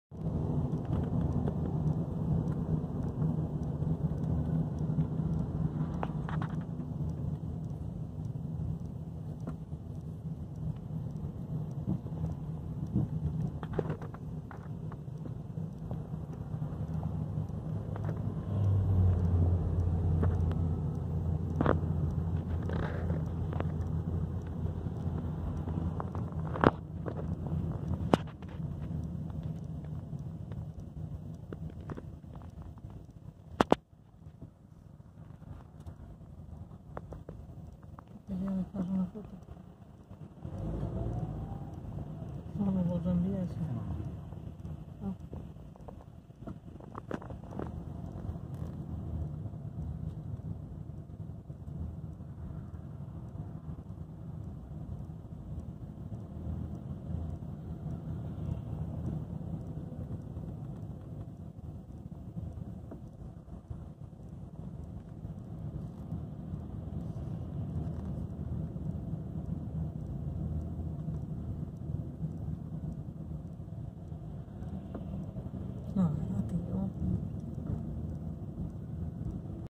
Car driving in the middle of two lanes
This person was driving in the middle of two lanes for no reason during a traffic jam.